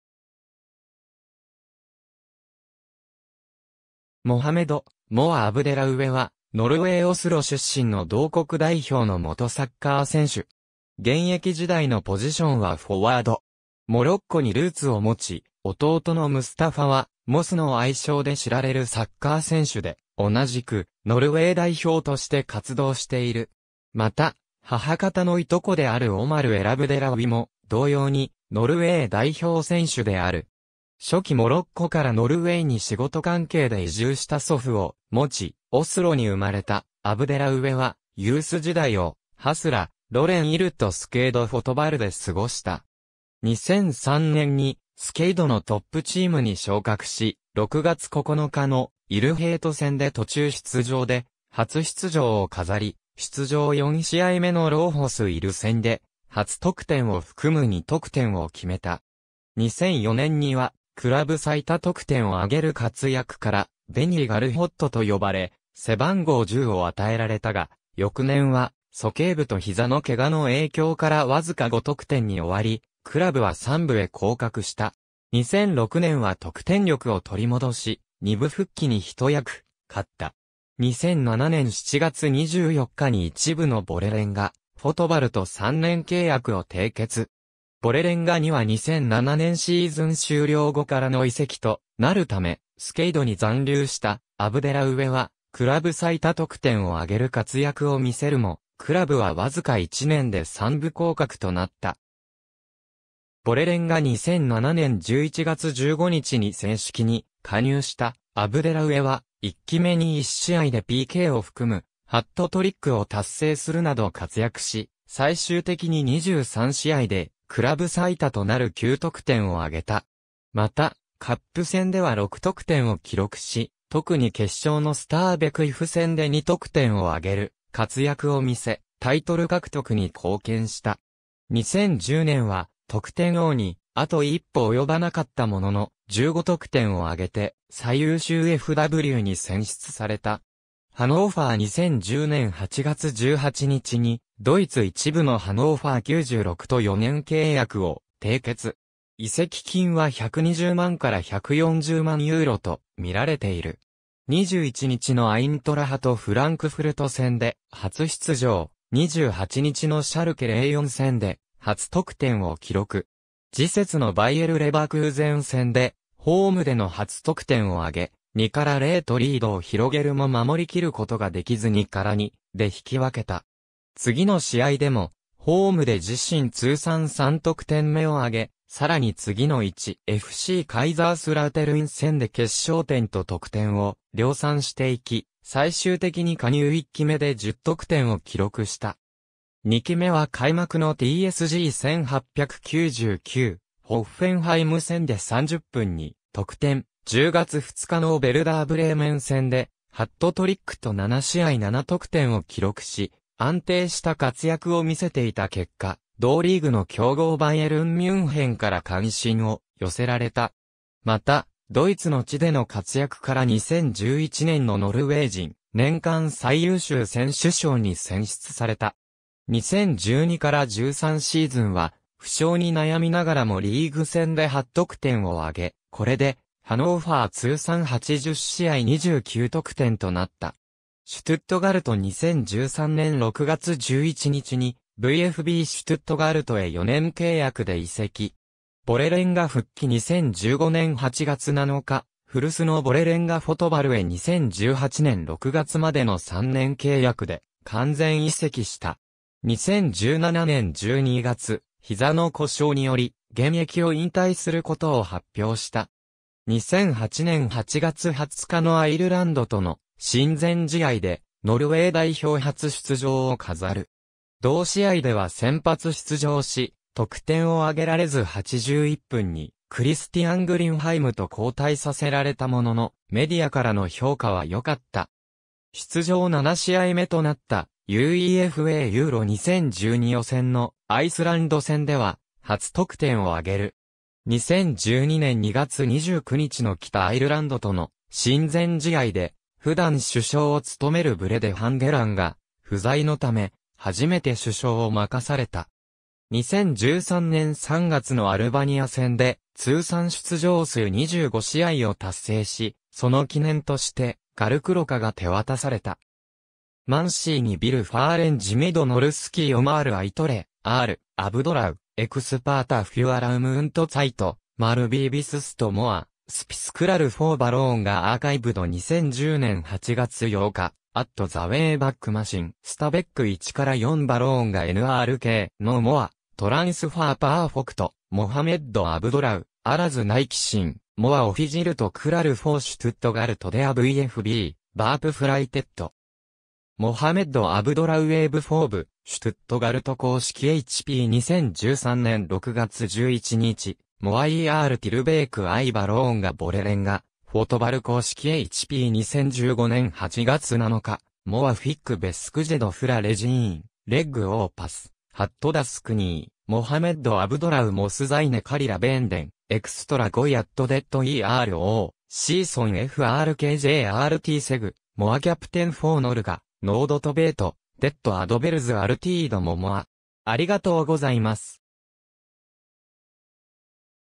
モハメド・アブデラウエ
スケイド・フォトバルの選手
ヴォレレンガ・フォトバルの選手
モハメド "モア"・アブデラウエ（Mohammed "Moa" Abdellaoue, 1985年10月23日- ）は、ノルウェー・オスロ出身の同国代表の元サッカー選手。
モロッコにルーツを持ち、弟のムスタファ（en）は"Mos"の愛称で知られるサッカー選手で、同じくノルウェー代表として活動している。
また、母方の従兄弟であるオマル・エラブデラウィも同様にノルウェー代表選手である。
初期 モロッコからノルウェーに仕事関係で移住した祖父を持ち、オスロに生まれたアブデラウエはユース時代をHasle-Løren ILとスケイド・フォトバルで過ごした。
2003年にスケイドのトップチームに昇格し、6月9日のILヘート（en）戦で途中出場で初出場を飾り、出場4試合目のRaufoss IL戦（2-1勝利）で初得点を含む2得点を決めた。
2004年にはクラブ最多得点を挙げる活躍から"Benny Gullfot"と呼ばれ、背番号10を与えられたが、翌年は鼠径部と膝の怪我の影響から僅か5得点に終わり、クラブは3部へ降格した。
2006年は得点力を取り戻し、2部復帰に一役買った。
2007年7月24日に1部のヴォレレンガ・フォトバルと3年契約を締結。
ヴォレレンガには2007年シーズン終了後からの移籍となるため、スケイドに残留したアブデラウエはクラブ最多得点を挙げる活躍を見せるも、クラブは僅か1年で3部降格となった。
ヴォレレンガ 2007年11月15日に正式に加入したアブデラウエは、1季目に1試合でPKを含むハットトリックを達成するなど活躍し、最終的に23試合でクラブ最多となる9得点を挙げた。
また、カップ戦では6得点を記録し、特に決勝のスターベクIF戦（4-1勝利）で2得点を挙げる活躍を見せ、タイトル獲得に貢献した。
2010年は得点王（16得点）にあと一歩及ばなかったものの、15得点を挙げて最優秀FWに選出された。
ハノーファー 2010年8月18日にドイツ1部のハノーファー96と4年契約を締結。
移籍金は120万から140万ユーロと見られている。
21日のアイントラハト・フランクフルト戦（ホーム2-1勝利）で初出場、28日のシャルケ04戦（アウェイ2-1勝利）で初得点を記録。
次節のバイエル・レバークーゼン戦でホームでの初得点を挙げ、2-0とリードを広げるも守りきることが出来ず2-2で引き分けた。
次の試合でも、ホーム（4-1勝利）で自身通算3得点目を挙げ、さらに次の1.FCカイザースラウテルン戦で決勝点と得点を量産していき、最終的に加入1季目で10得点を記録した。
2季目は開幕のTSG1899ホッフェンハイム戦で30分に得点、10月2日のヴェルダー・ブレーメン戦（3-2勝利）でハットトリックと7試合7得点を記録し、安定した活躍を見せていた結果、同リーグの強豪バイエルン・ミュンヘンから関心を寄せられた。
また、ドイツの地での活躍から2011年のノルウェー人年間最優秀選手賞に選出された。
2012-13シーズンは負傷に悩みながらもリーグ戦で8得点を挙げ、これでハノーファー通算80試合29得点となった。
シュトゥットガルト 2013年6月11日にVfBシュトゥットガルトへ4年契約で移籍。
ヴォレレンガ復帰 2015年8月7日、古巣のヴォレレンガ・フォトバルへ2018年6月までの3年契約で完全移籍した。
2017年12月、膝の故障により現役を引退することを発表した。
2008年8月20日のアイルランドとの親善試合（1-1）でノルウェー代表初出場を飾る。
同試合では先発出場し、得点を挙げられず81分にクリスティアン・グリンハイムと交代させられたものの、メディアからの評価は良かった。
出場7試合目となったUEFA EURO 2012予選のアイスランド戦では初得点を挙げる。
2012年2月29日の北アイルランドとの親善試合で普段主将を務めるブレデ・ハンゲランが不在のため、初めて主将を任された。
2013年3月のアルバニア戦で通算出場数25試合を達成し、その記念としてGullklokkaが手渡された。
^ "Mancini vil forlenge med norske Omar i...